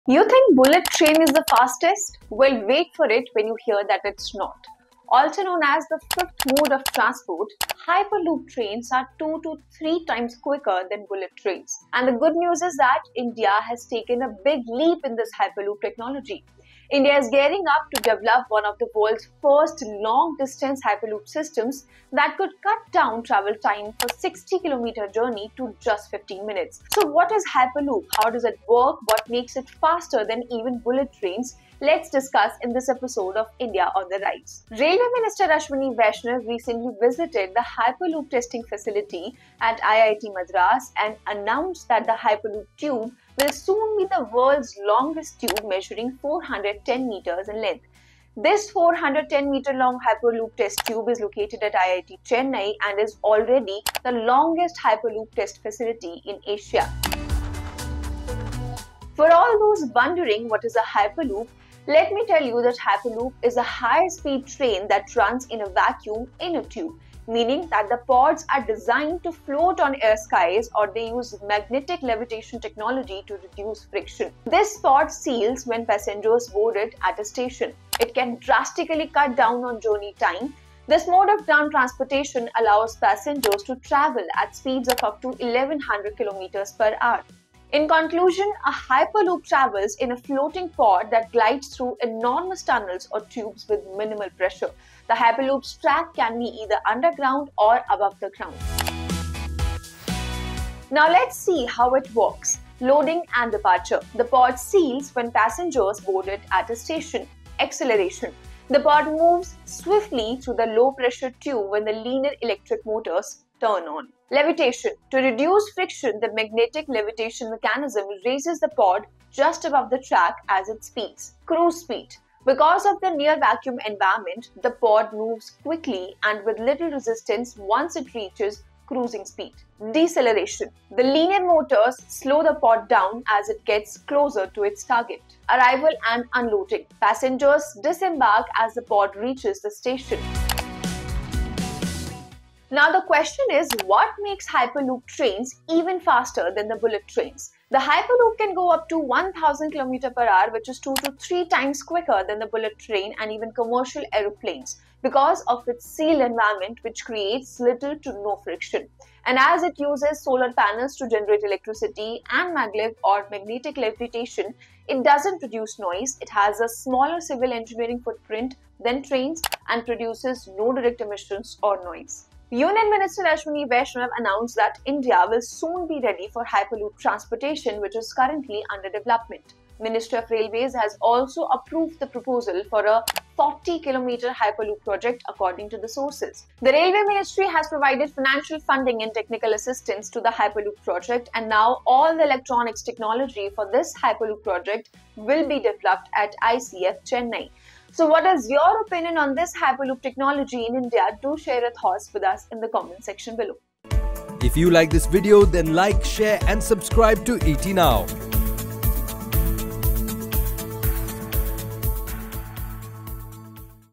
Asia's Longest Hyperloop Track In India! What Is Hyperloop? Is It Faster Than Bullet Train?
India has taken a big leap in this hyperloop technology. India is gearing up to develop one of the world’s first long-distance hyperloop systems. So what is a hyperloop? How does it work? What makes it faster than even bullet trains? Watch to know.